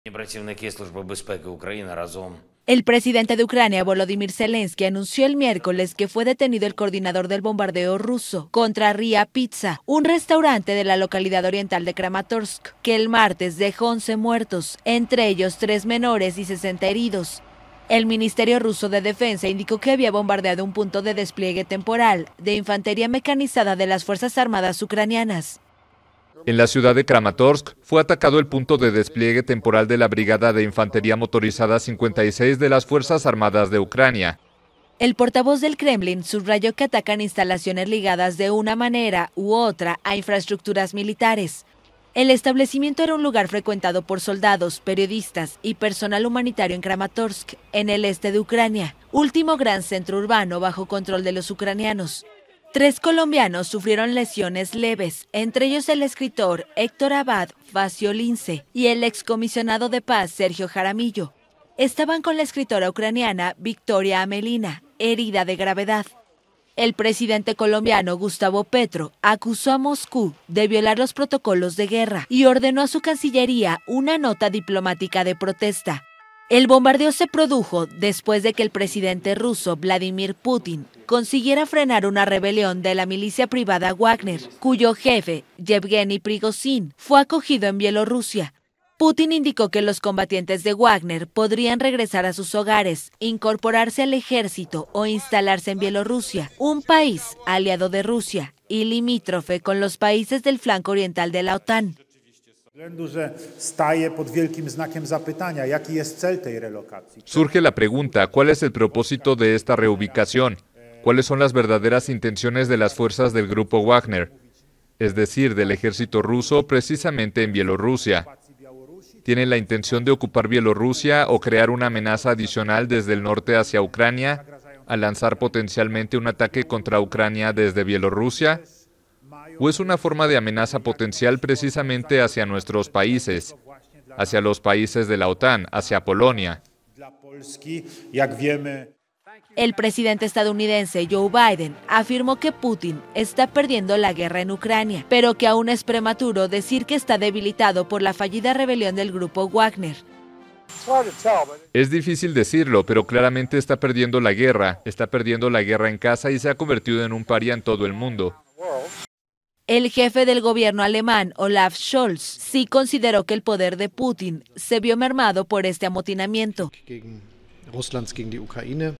Ucrania anunció arresto de "coordinador" del bombardeo ruso contra restaurante
El presidente ucraniano, Volodimir Zelenski, indicó el miércoles que los servicios de seguridad detuvieron al "coordinador" del bombardeo ruso contra un restaurante de la ciudad oriental de Kramatorsk, que el martes dejó 11 muertos y 60 heridos y provocó una ola de indignación internacional.